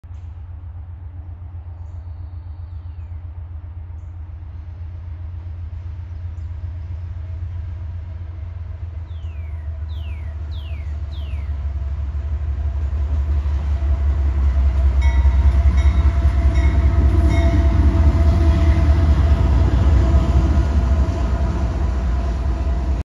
CSXT U217
Light motors head north. First time hearing a U symbol since the CSX Symbols was put into current format